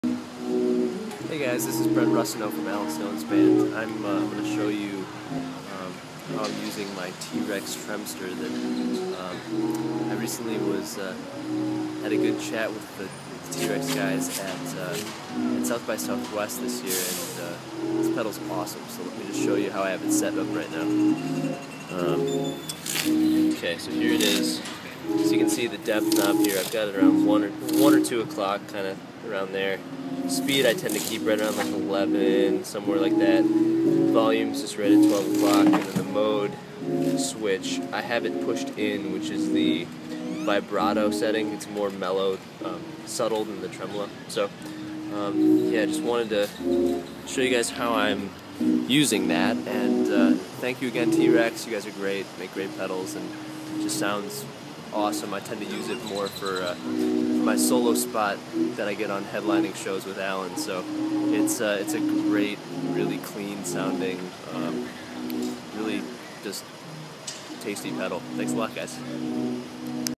Brent Rusinow (Allen Stone's bass player) talks about the T Rex Tremster
Brent explains why he loves this tremolo pedal.  Thanks to John Kelley at Musiquip (T Rex's US Distributor)!